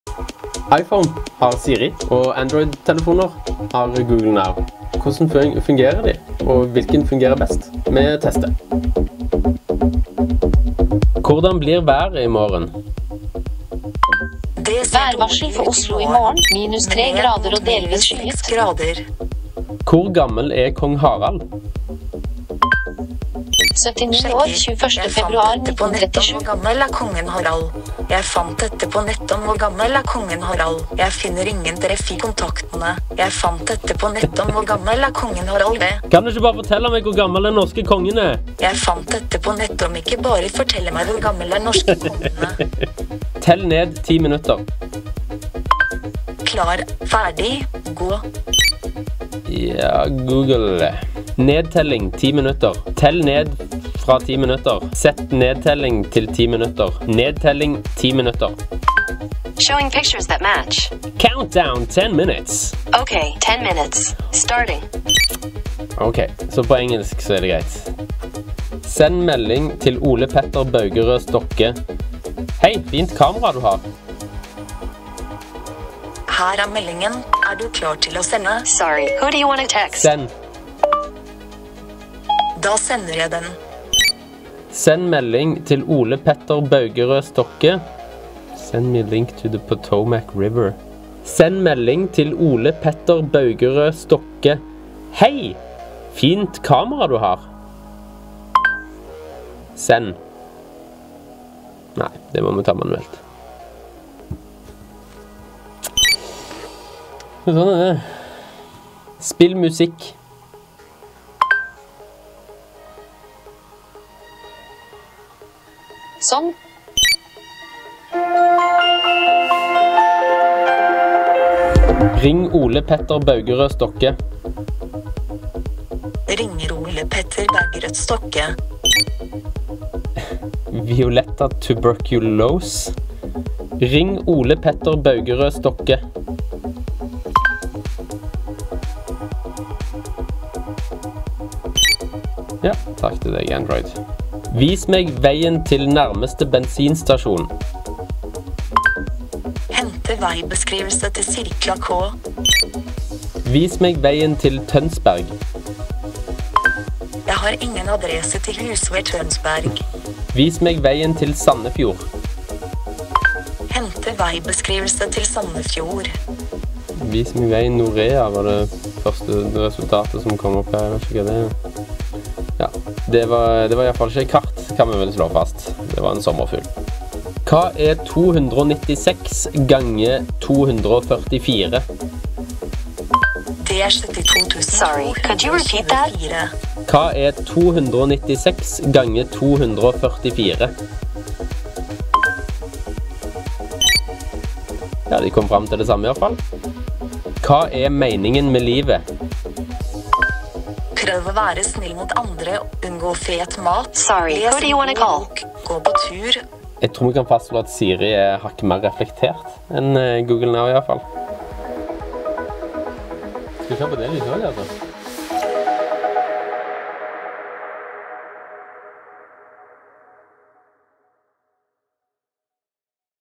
iPhones Siri vs. Androids Google Now på norsk
Vi tester hvor flinke Siri på iPhone og Google Now på Android er til å forstå norsk. Poengsummen ble likere enn vi hadde trodd!